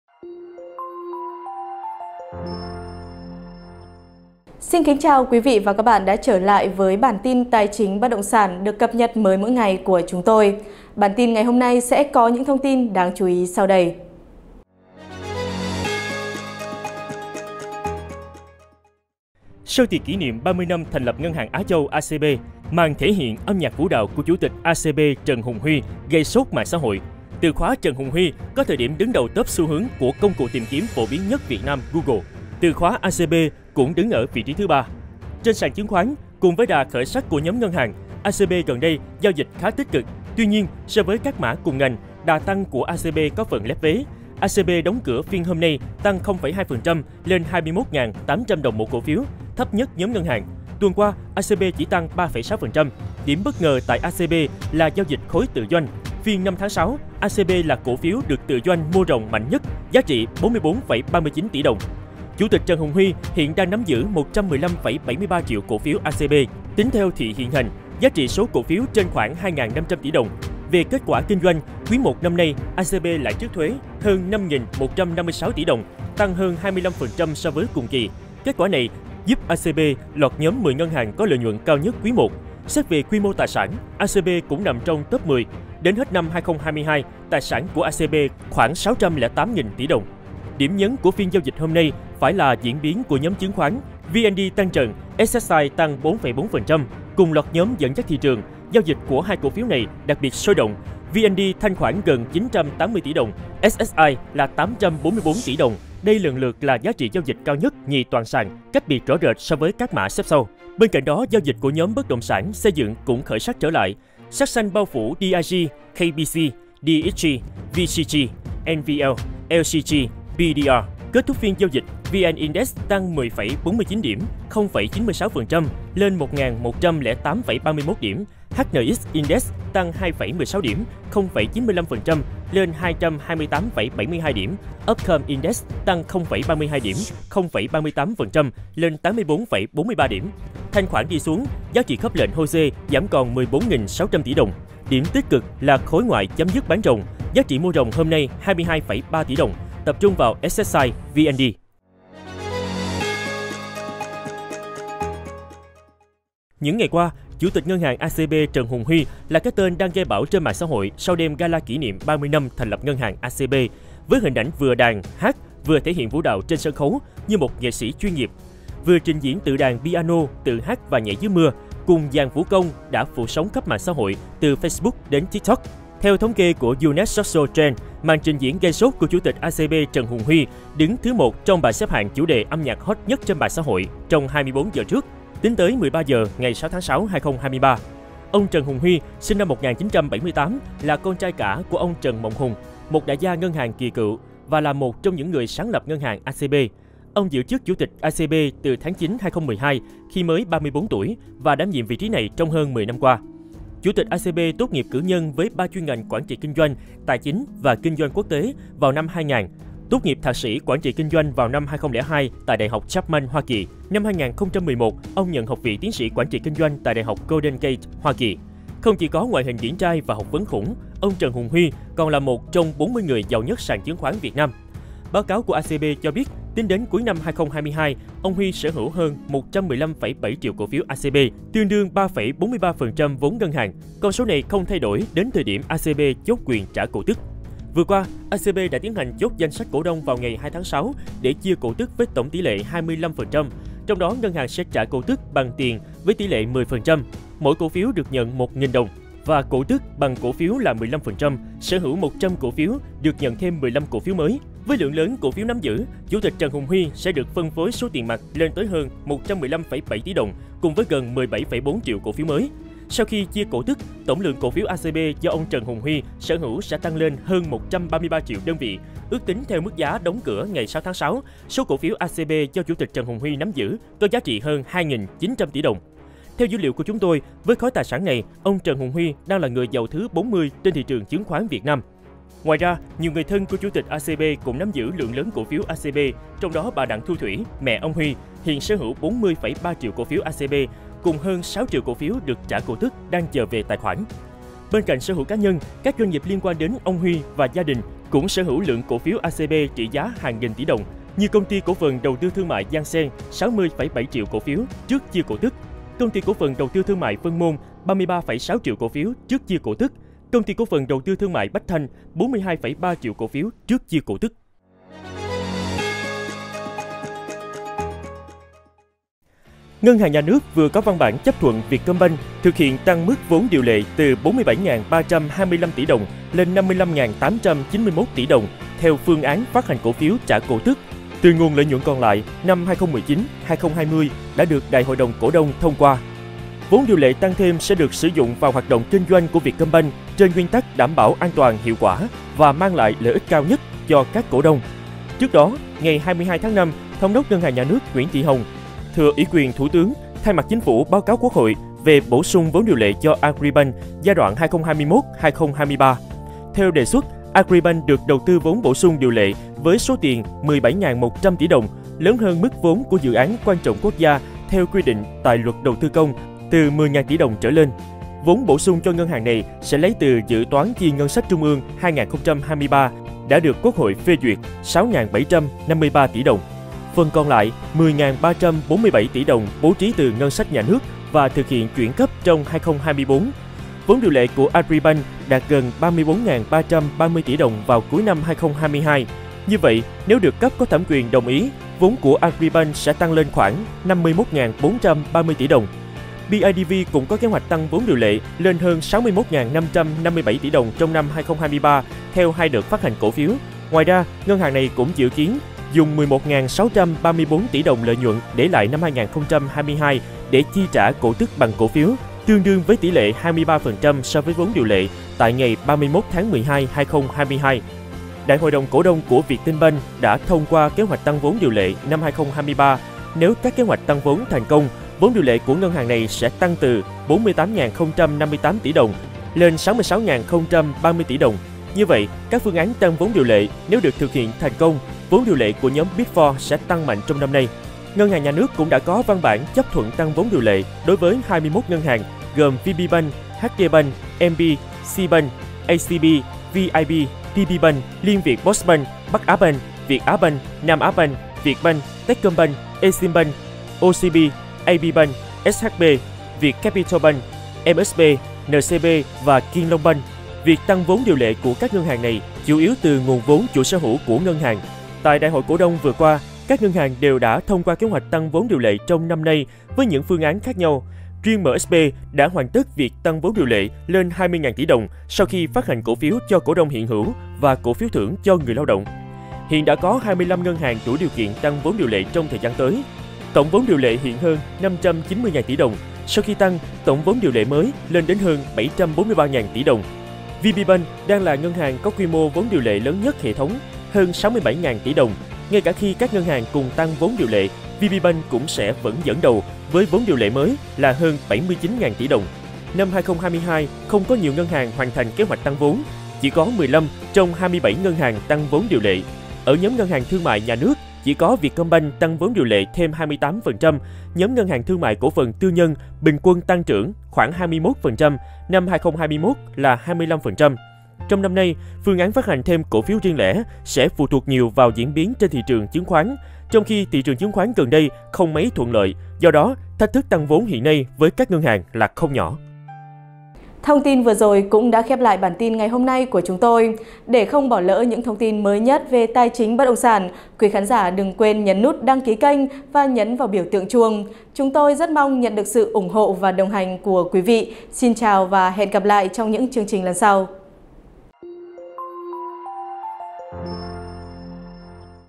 Chủ tịch "soái ca" của ACB giàu cỡ nào mà khiến cổ phiếu biến động sau màn nhảy gây sốt? | CafeLand
Chủ tịch "soái ca" của ACB giàu cỡ nào mà khiến cổ phiếu biến động sau màn nhảy gây sốt?

Sau tiệc kỷ niệm 30 năm thành lập Ngân hàng Á Châu (ACB), màn thể hiện âm nhạc, vũ đạo của chủ tịch ACB Trần Hùng Huy gây “sốt” mạng xã hội. Từ khóa “Trần Hùng Huy” có thời điểm đứng đầu top xu hướng của công cụ tìm kiếm phổ biến nhất Việt Nam - Google. Từ khoá “ACB” cũng đứng ở vị trí thứ 3.